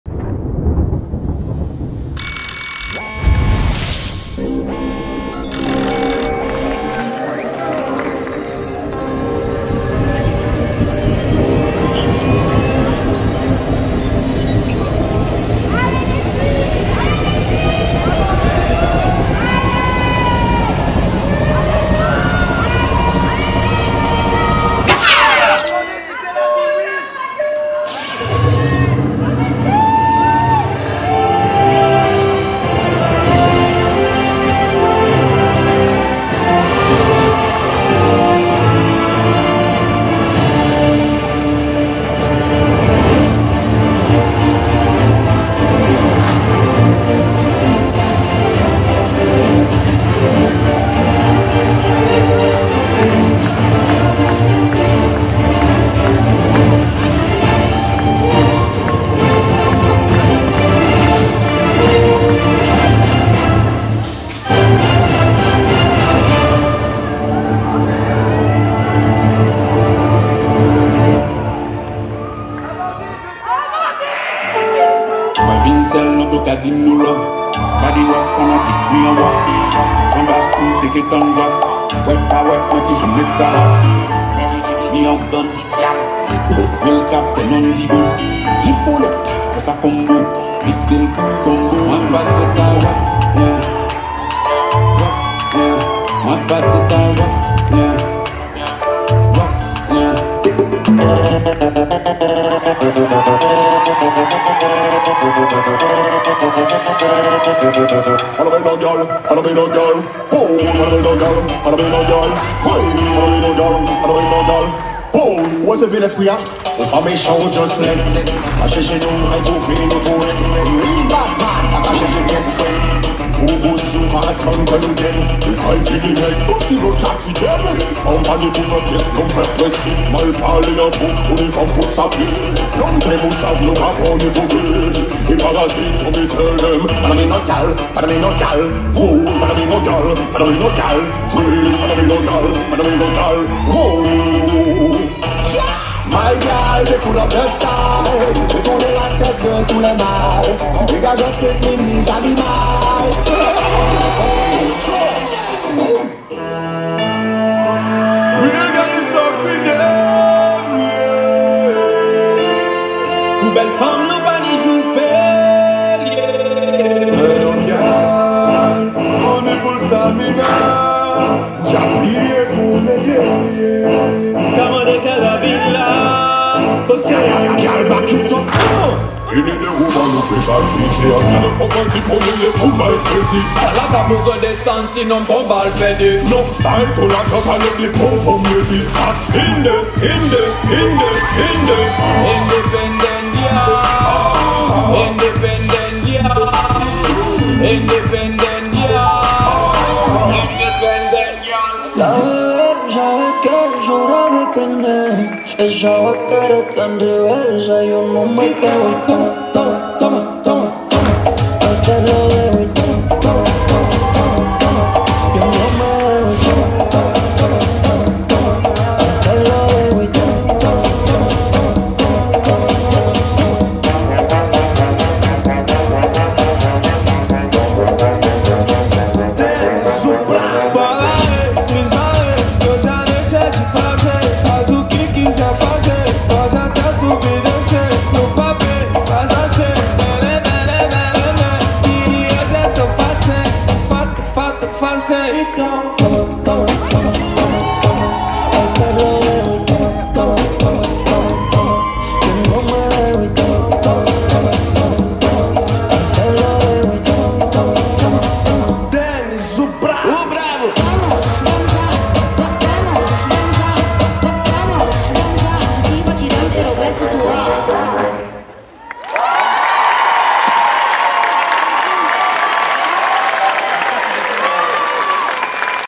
BACK to SCHOOL RAG' - 1718 La Piraterie
Show de STREET RAGGA chorégraphié par Yaëlle Doucet pour le spectacle 2024 de School Rag'.